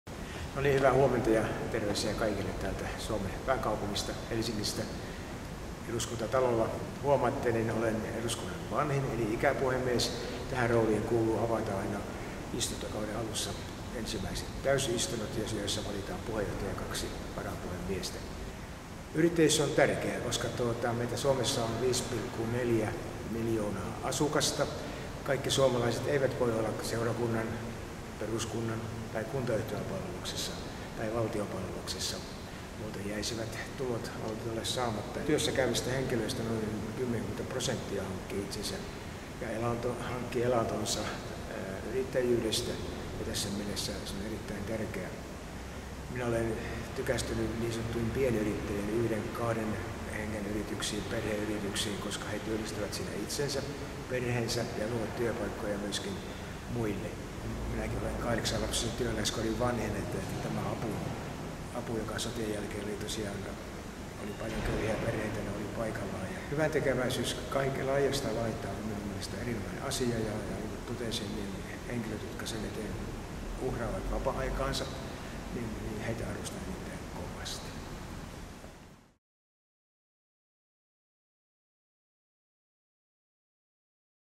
Antais.in - Kauko Tuupainen
Kauko ottaa kantaa avoimesti yrittäjyyteen ja yhteiskuntarakenteeseen.